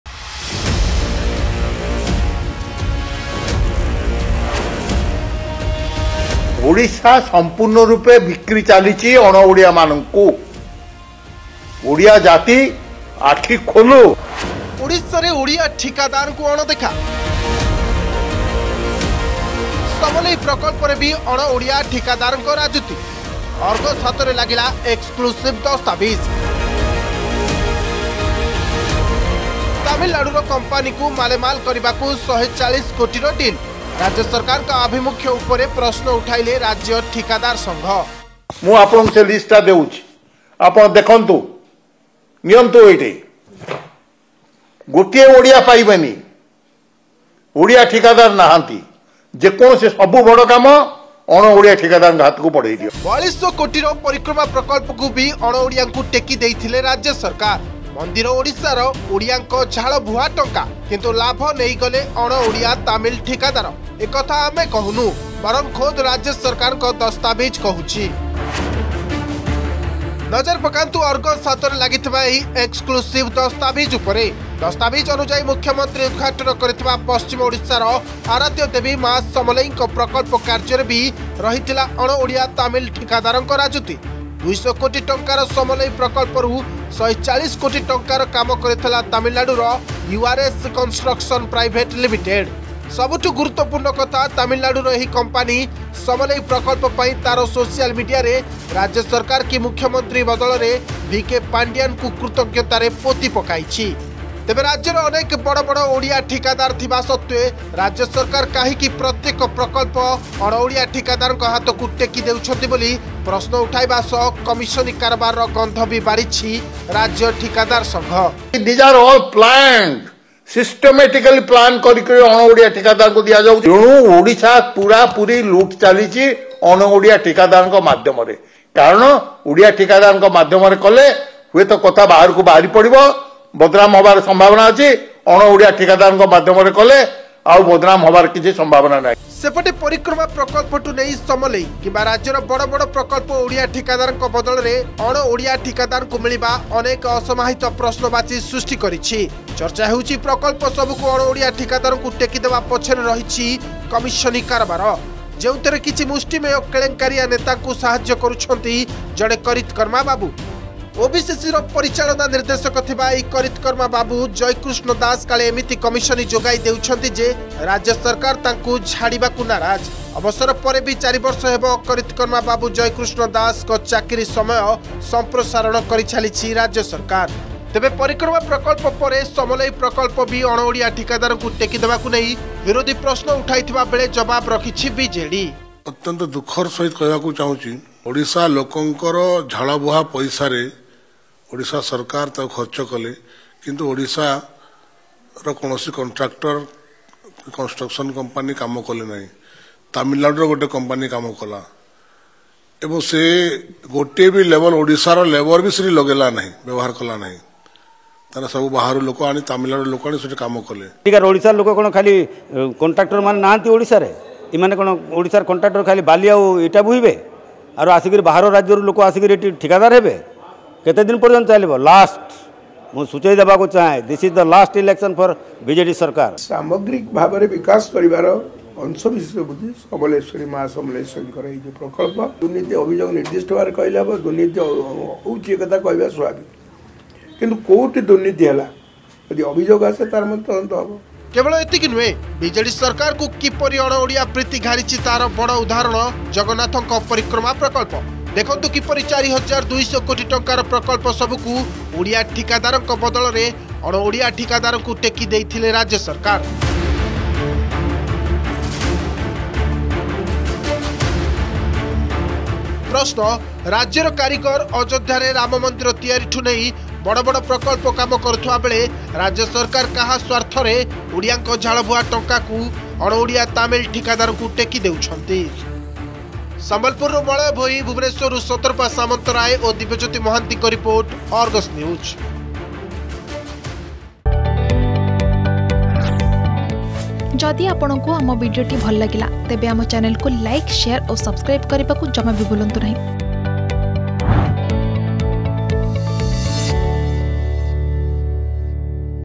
ସମଲେଇ ପ୍ରକଳ୍ପରେ ବି ଅଣଓଡିଆ ଠିକାଦାରଙ୍କ ରାଜୁତି || Samalei Project
ଓଡିଶାରେ ଓଡିଆ ଠିକାଦାରଙ୍କୁ ଅଣଦେଖା । ସମଲେଇ ପ୍ରକଳ୍ପରେ ବି ଅଣଓଡିଆ ଠିକାଦାରଙ୍କ ରାଜୁତି । ଅର୍ଗସ ହାତରେ ଲାଗିଲା EXCLUSIVE ଦସ୍ତାବିଜ୍ । ତାମିଲନାଡୁର କମ୍ପାନୀକୁ ମାଲେମାଲ କରିବାକୁ ୧୪୦ କୋଟିର ଡିଲ୍ । ରାଜ୍ୟ ସରକାରଙ୍କ ଆଭିମୁଖ୍ୟ ଉପରେ ପ୍ରଶ୍ନ ଉଠାଇଲେ ରାଜ୍ୟ ଠିକାଦାର ସଙ୍ଘ ।